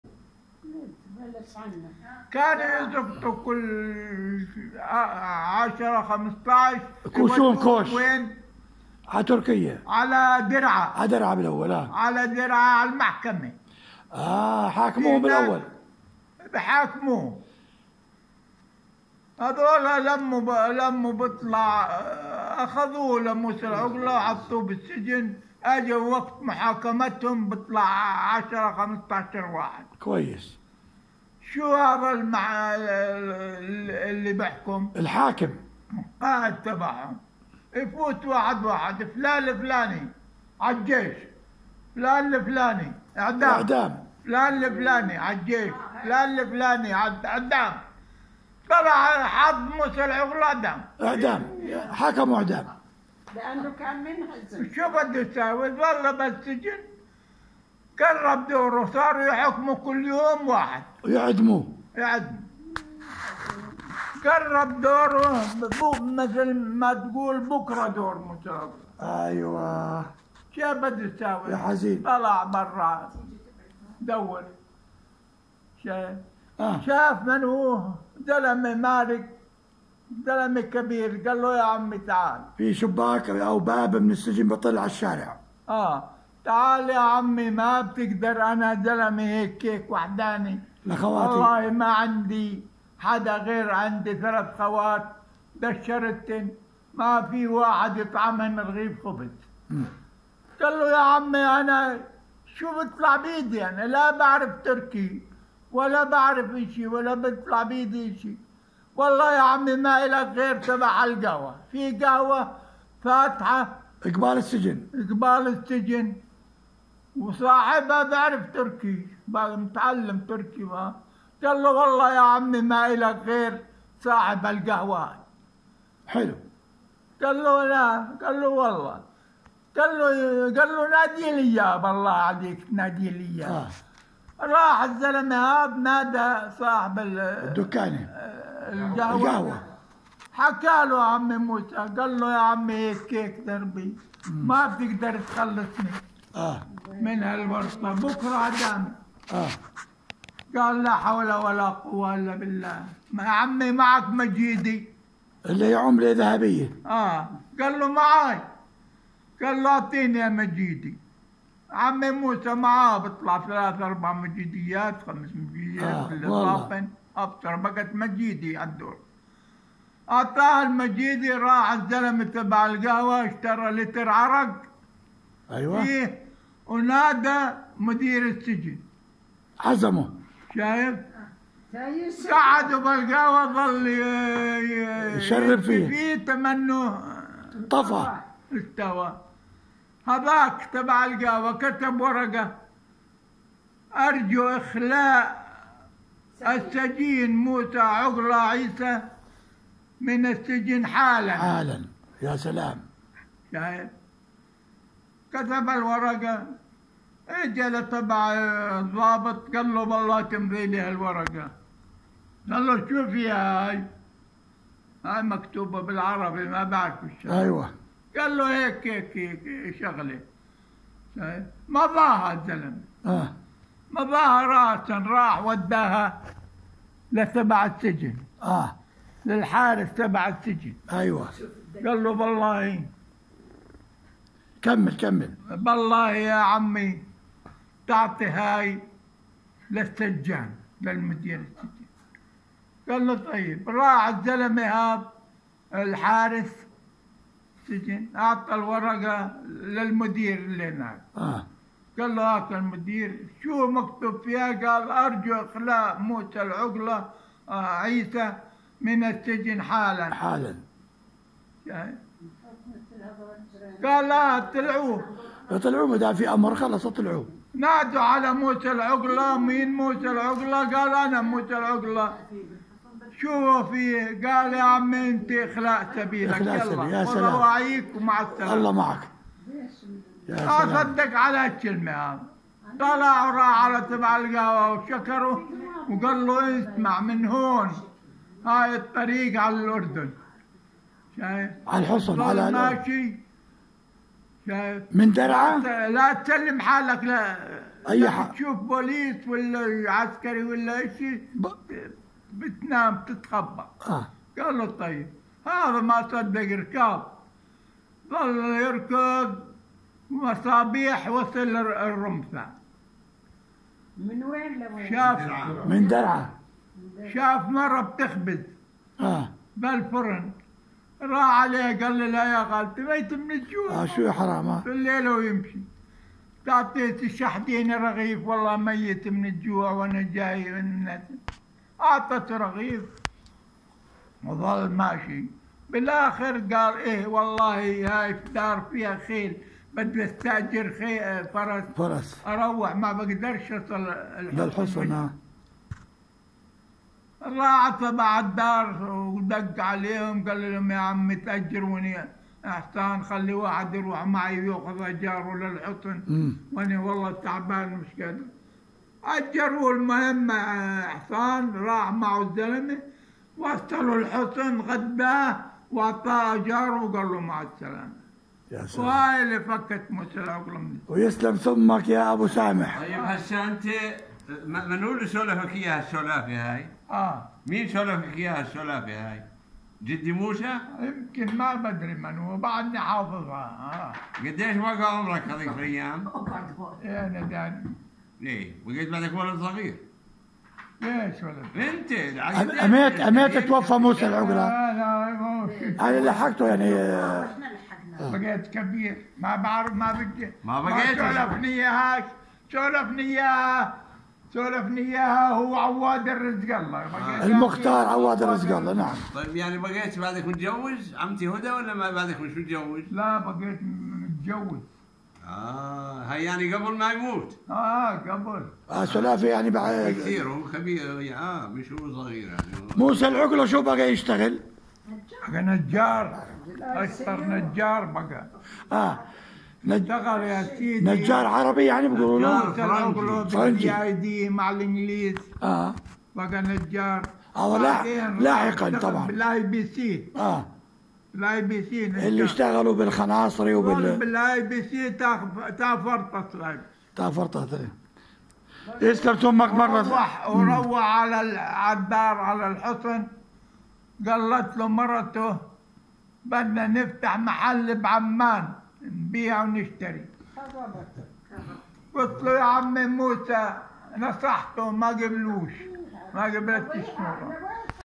سواليف حصناوية عتيقة يرويها ناصر عيسى النمري : كيف نجى موسى العقلة من الاعدام..
سواليف حصناوية عتيقة.. يرويها العم ابو سامح  /  ناصر عيسى النمري ..  وهو ما شالله من المعمرين القلائل في الحصن وقد اتم المائة عام من عمره المديد انشالله.. وقد احتفل بعيد ميلاده المائة قبل ايام وهو من مواليد عام 1924..
في هذا الفيديو يروي لنا قصة عمه / والدة زوجته / موسى العقلة النمري / ابو سامي الذي كاد ان يتم اعدامه من قبل الاتراك على ايام السفر برلك بسبب عدم التحاقه بالخدمة العسكرية بالجيش العصملي وقد وحيد اهله ولم  يكن له سوى ثلات خوات بنات وهو الذي يعيلهن... ولم يشفع له له لدى الاتراك سوى حيلة وفطنة صاحب قهوة قريبة من السجن في مدينة درعا...
اليكم السالفة من طقطق للسلام  عليكوا..